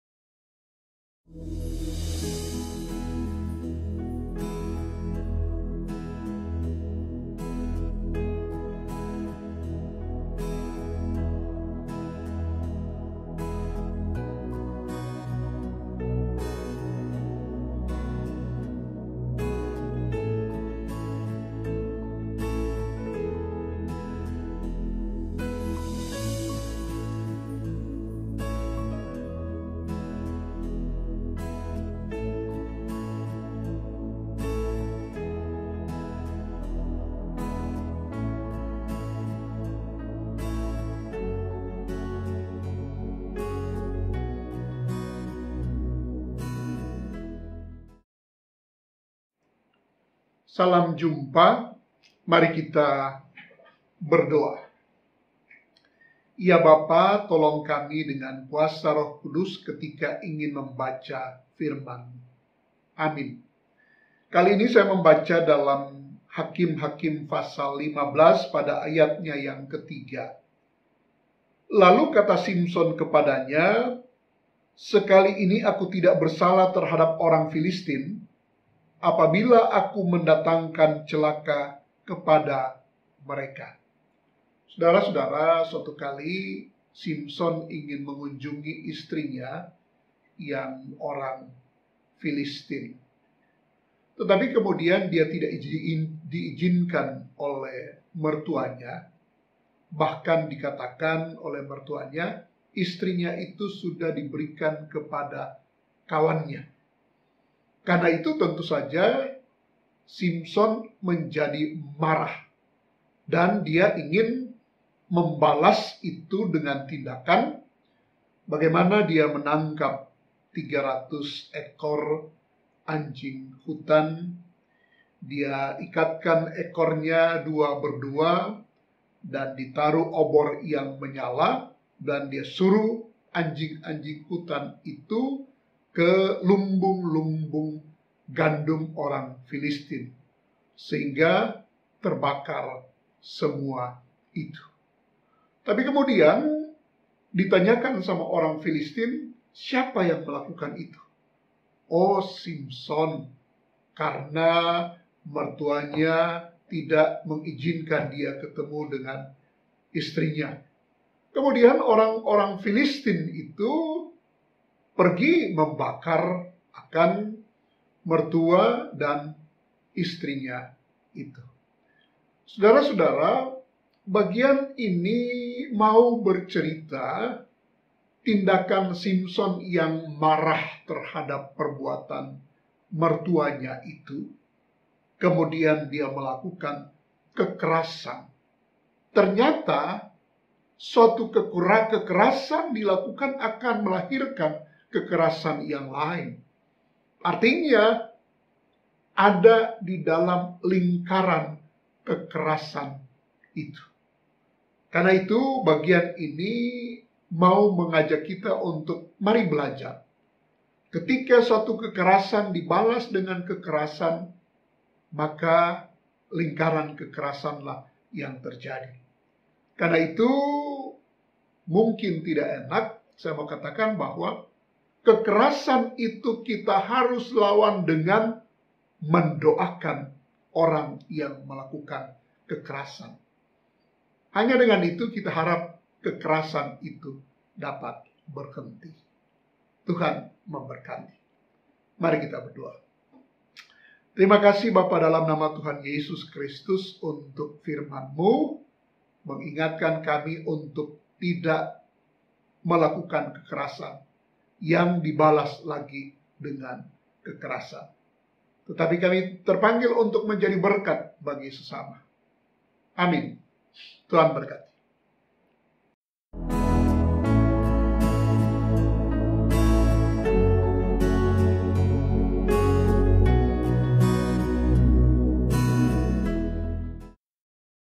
(GPIB Gloria) Renungan Pagi - Sabtu, 11 Juli 2020
Sabda Bina Umat (SBU) Pagi GPIB
"Tidak Membalas Kekerasan, Namun Mendoakan Pelaku Kekerasan "
Hakim-Hakim 11 : 2

Pelayan Firman:
Pdt. Jan Jona Lumanauw

Backsound Created By:
Ron Mascarenhas